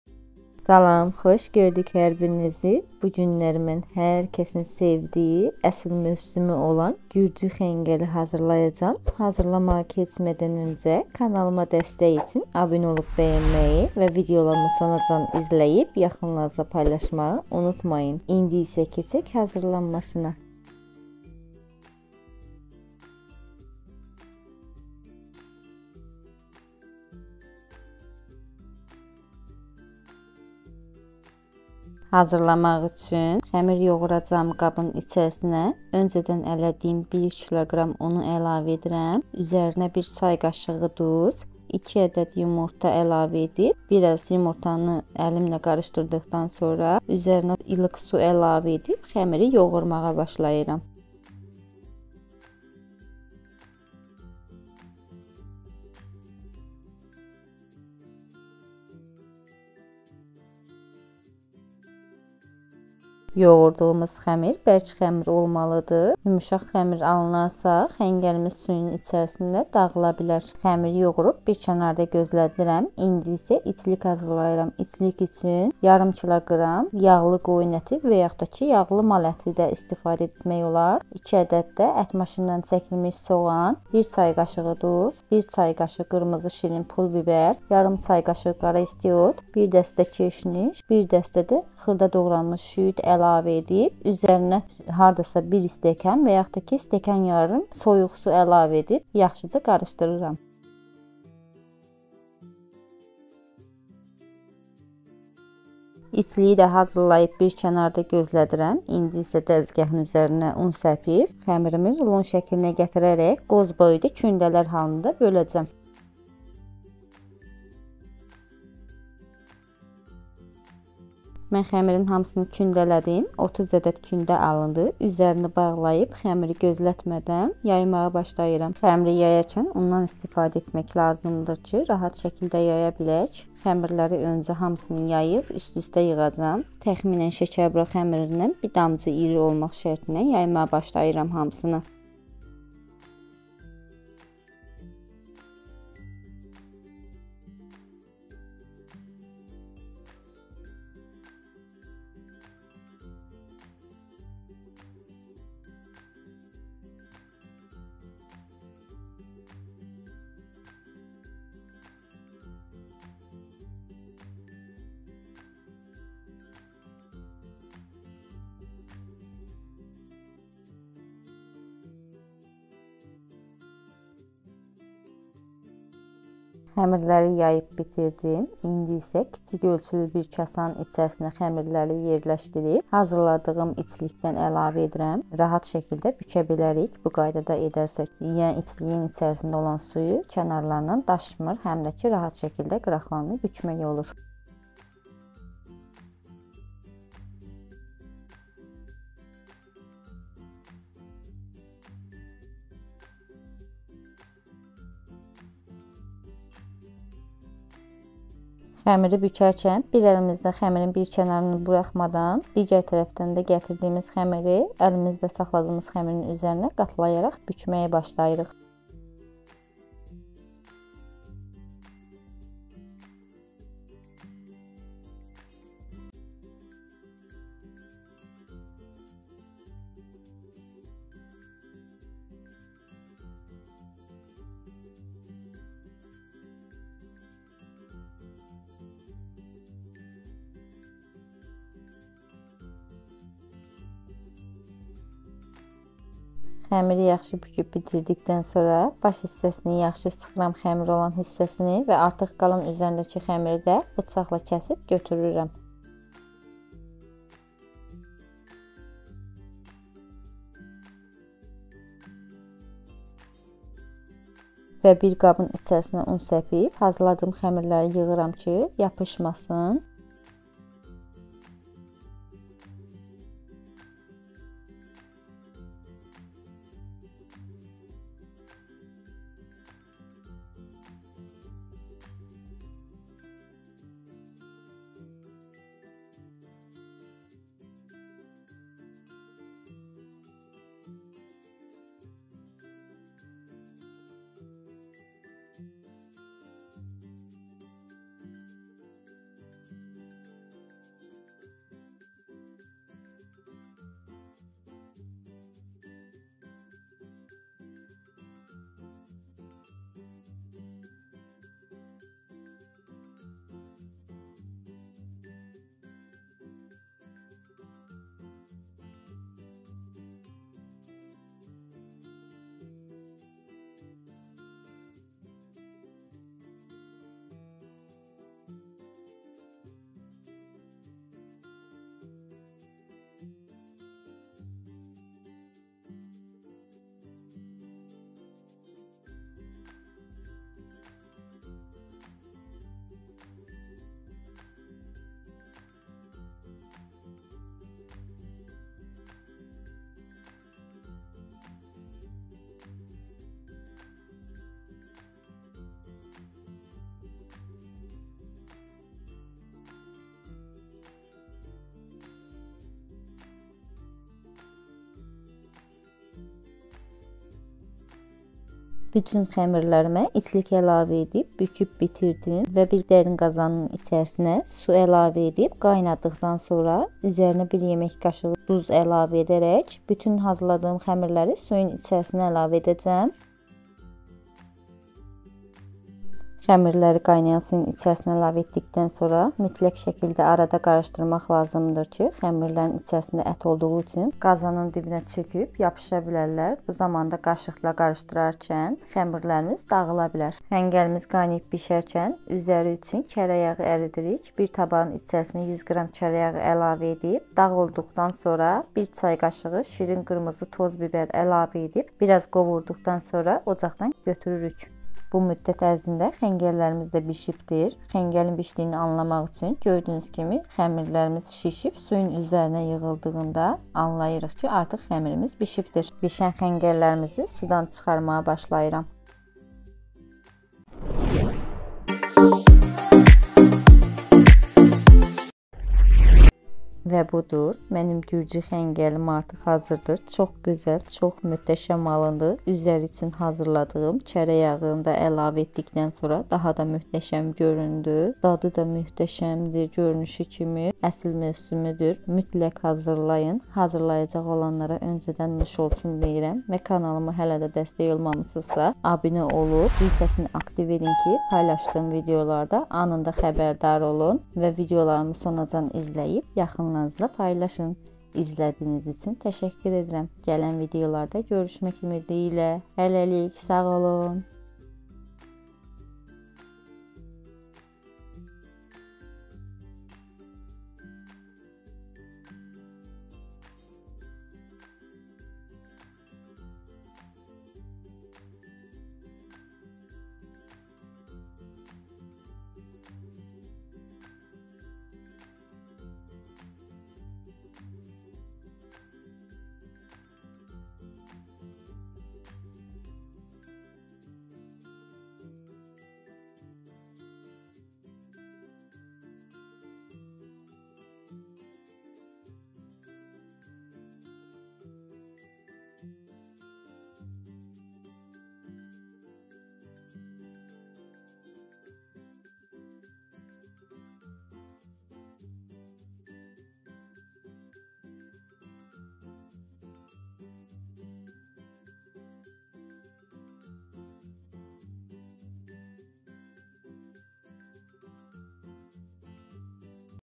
dadlı ləzzətli gürcü xengeli əsil mövsümüdür mütləq hazırlayın
Resepti
Xəmiri
1. Kg un
2. Ədəd yumurta
1. Çay qaşığı duz
İlıq su
İçlik
500.qr yağlı qiymə
2. Ədəd soğan
1. Çay qaşığı duz
1. Çay qaşığı qırmızı şirin toz bibər
0.5 Çay qaşığı istiot
1. Dəsdə şüyüd
1.Dəsdə keşniş
Üzəri üçün Zövgə görə
100. Qr kərə yağı
1. Çay qaşığı qırmızı şirin toz bibər
Qara istiyit
Nanə
Qatığ